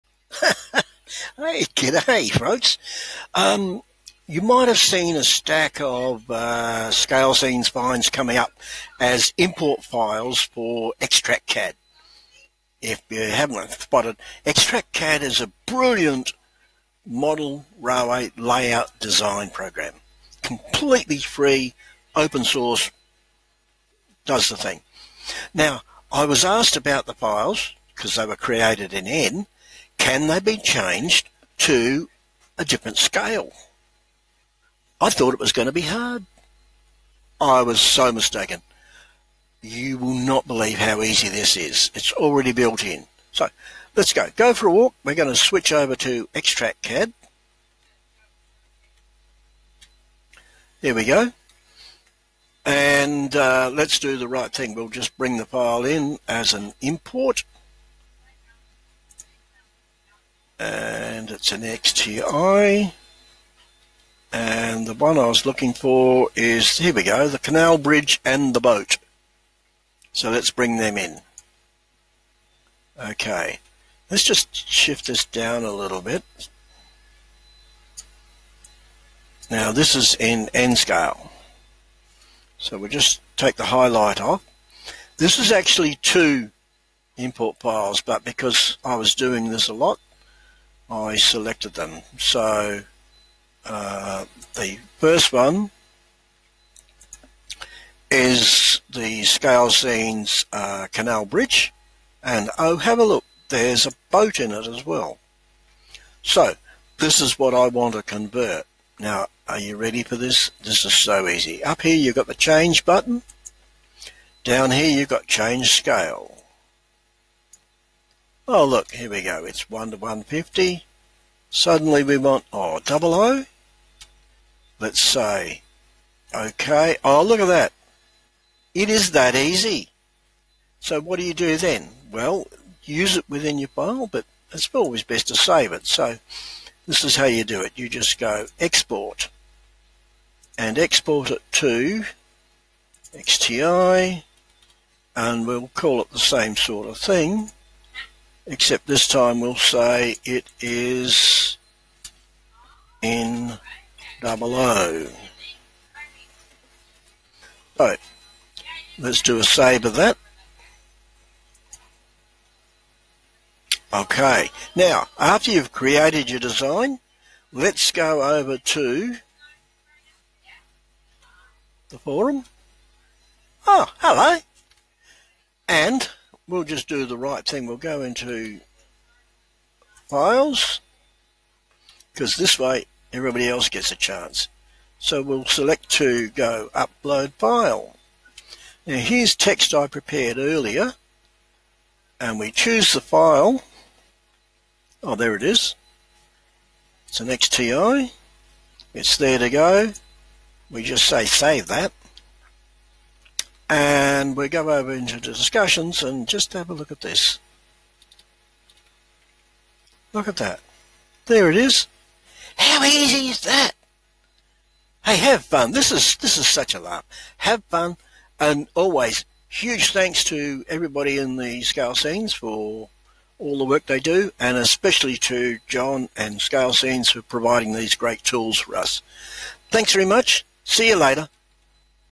Converting XTrkCAD Import files to another Scale - Scalescenes.com
I have created a stack of Import files with the outlines of ScaleScenes.com's Card scene products. This helps users of XTrkCAD to work out where to put their ScaleScenes products once they have made them.
I only work in N scale so that is what I have created these files in. But others have asked if I can provide them in other scales such as the very popular OO or HO (they are NOT the same!). I thought it was going to be hard to do. I could not have been more wrong. It is so easy as XTrkCAD is already set up to do exactly that task.
Simply Import the Import file  (.XTI) into your page. Select all the elements you want to convert. Click on the Change Tab. At the bottom of that menu click on the Change Scale item. Set the Scale to the desired new scale and click OK. So easy.
Then you can save the new file and even share it around so others get the benefit.

I get most of my card files from Scalescenes.com

This has been recorded thanks to a really cool App, Loom. It runs as an App within Chrome but also has a stand-alone App that runs On your computer.